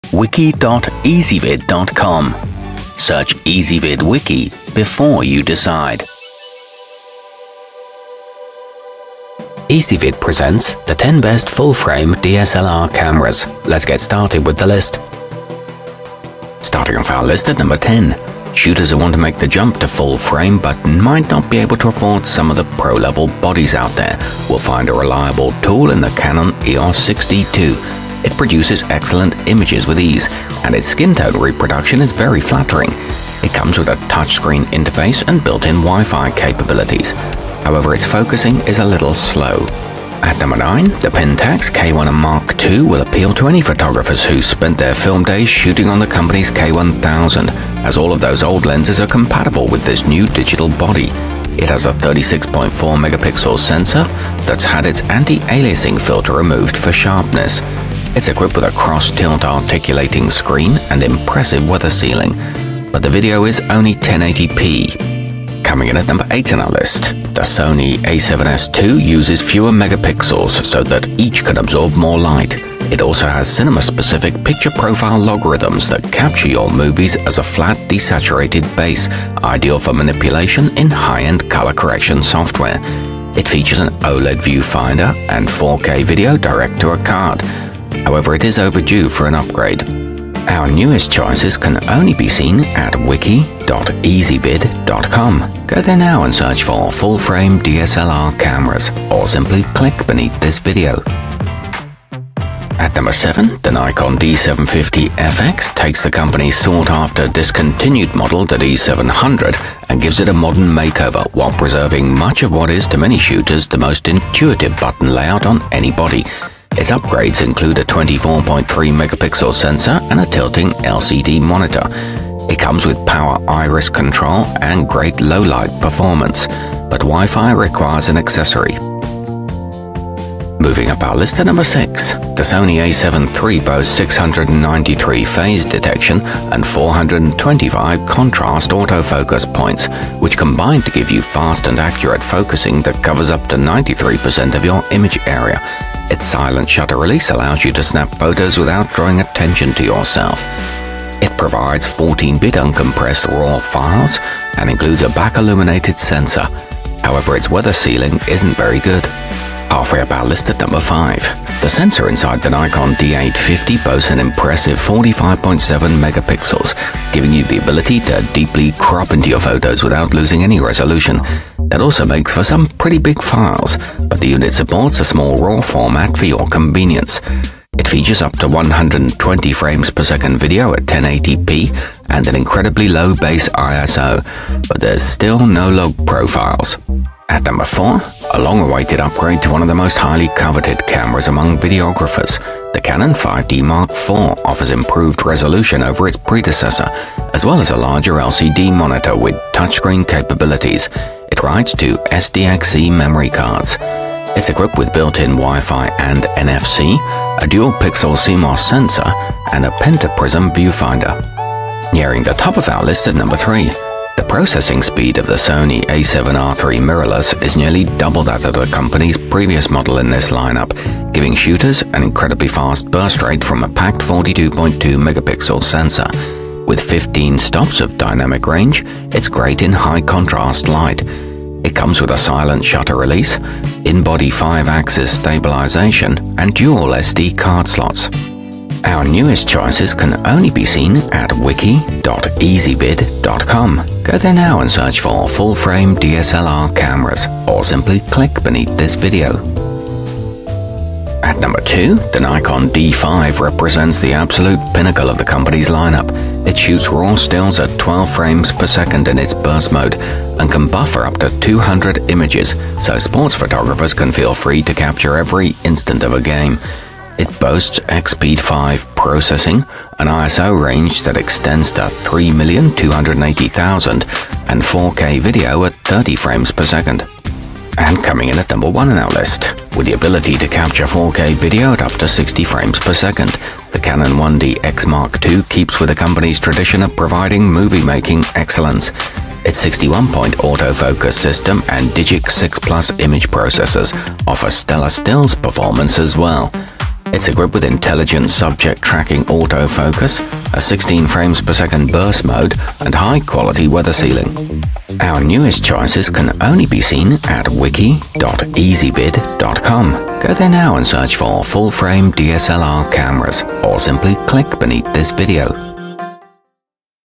10 Best Full-Frame DSLR Cameras 2018
Our complete review, including our selection for the year's best full-frame dslr camera, is exclusively available on Ezvid Wiki.

Full-frame dslr cameras included in this wiki include the canon 1d x mark ii, pentax k-1 mk ii, nikon d5, nikon d850, canon eos 6d ii, nikon d750 fx, canon 5d mark iv, sony a7r iii mirrorless, sony a7s ii, and sony a7 iii.